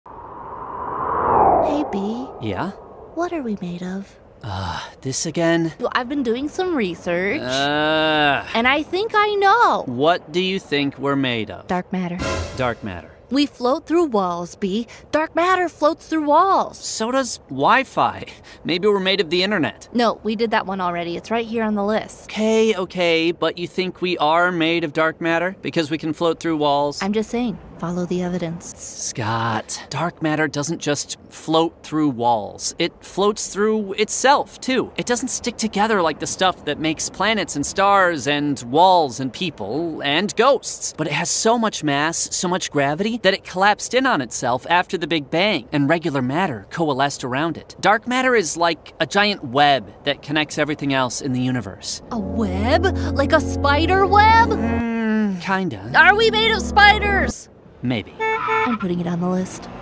Are Ghosts Made Of Dark Matter?
Two of the universe’s biggest questions: what are ghosts made of…and what is dark matter?